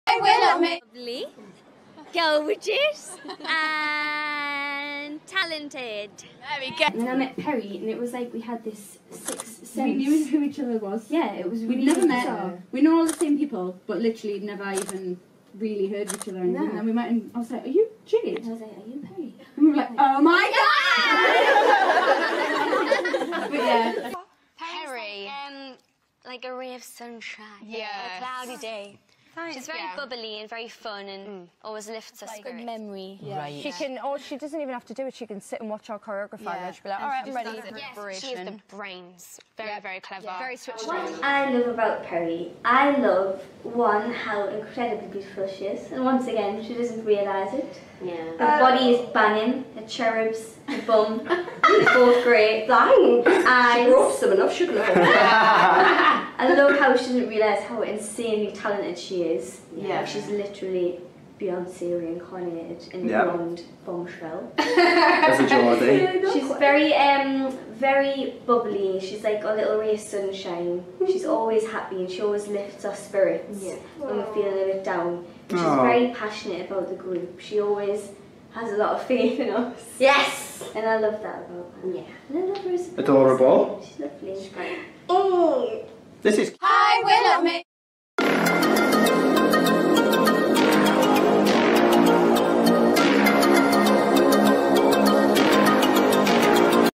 Jerrie intro
Just an introduction for my jerrie stuff (Pez and Jeed) Go on check it out on my list "jerrie stuff" to see jerrie update 2020 so far. Lots of love 💕